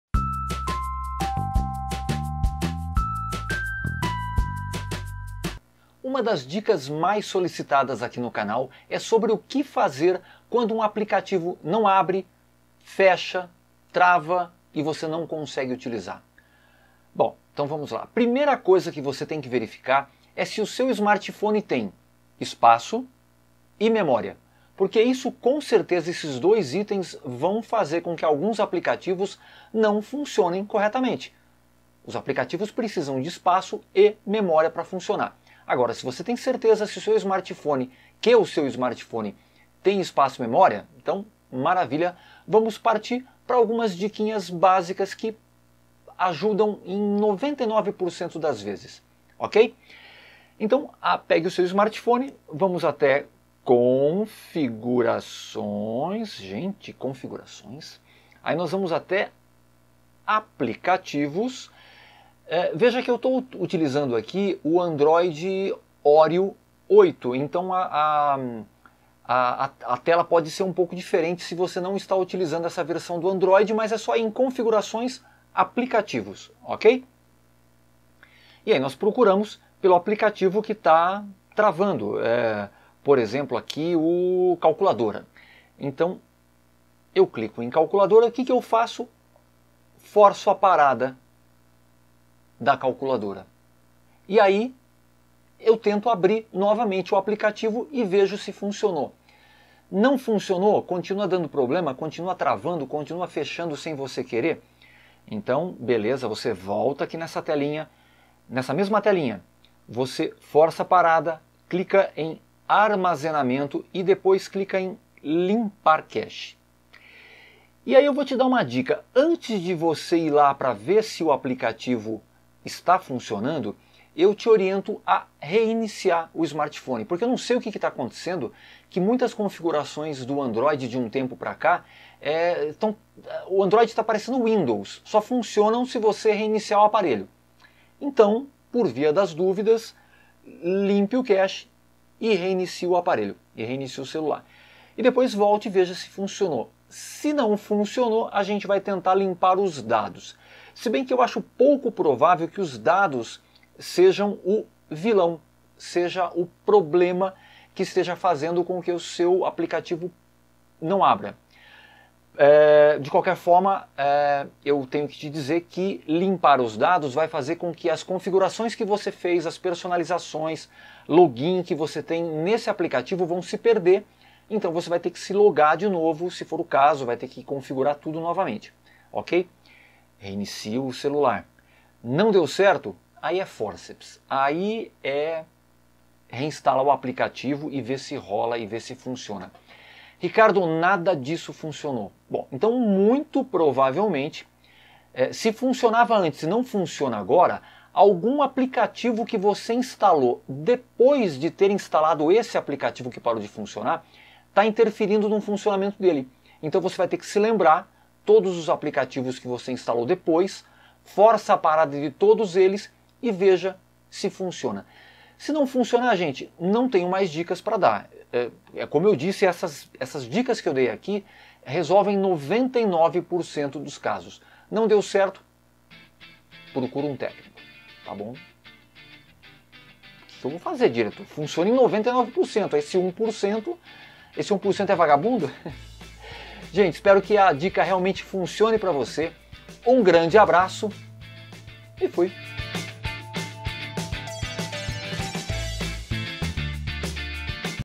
APLICATIVO ANDROID NÃO ABRE
A vídeo-dica de hoje vai te mostrar alguns métodos que vão resolver 99% dos problemas de travamento de aplicativos no Android.
Assista ao vídeo todo e não deixe de me fazer um carinho, deixando o seu JÓINHA!

Internet Lenta?
Use o celular, o PC, o Tablet e até a TV! Não precisa instalar nada!